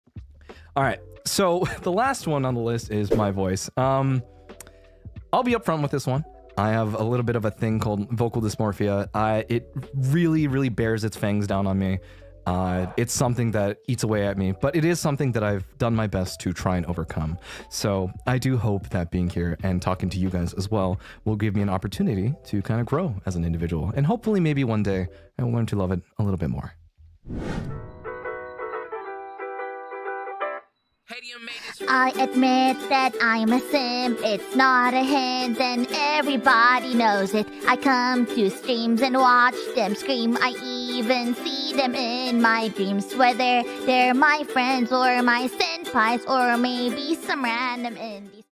Claude's talks on his Vocal Dysmorphia [Claude Clawmark| Nijisanji]
Be sure to check out Claude!
Big thanks to my Partner Inwa Noire for help In everything from gathering clips to the thumbnail! I couldn't do this without him!
Thanks more for liking!
Have a nice day!